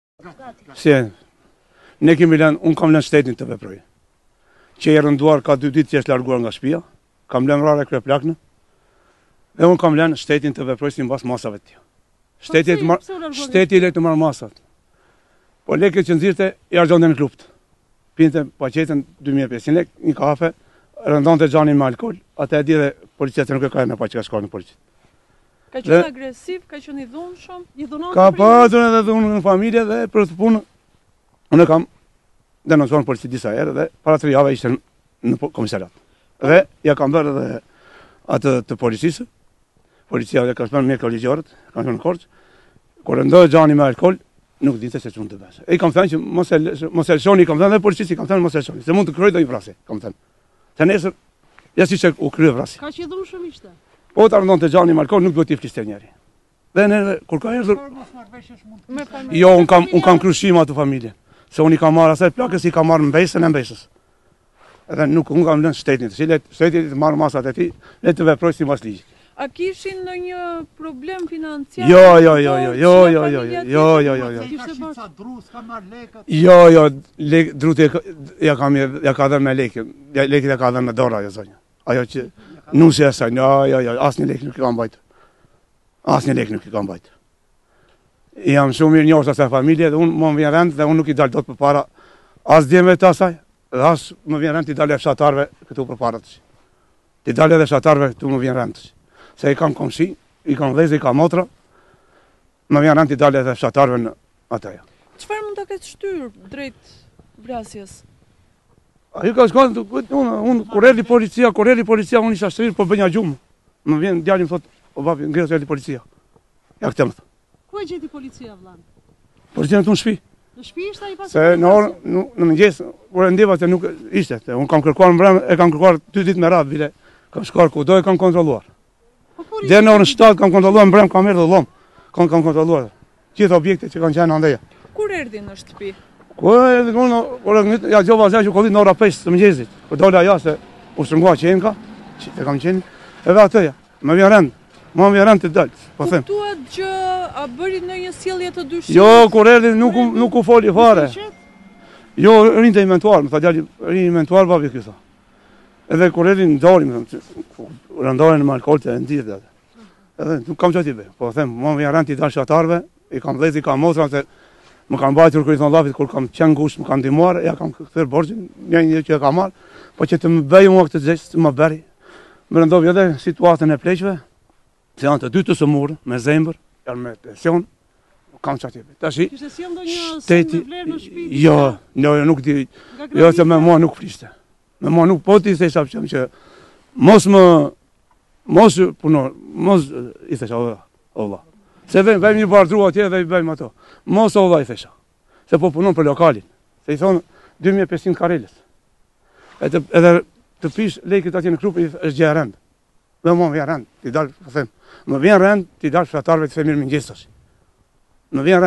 Vrau nusen dhe vjehrrën/I vëllai i autorit: Rrinte i pirë, po t’i fliste kush ai nuk mbante
Klikoni për të lexuar më shumë në linkun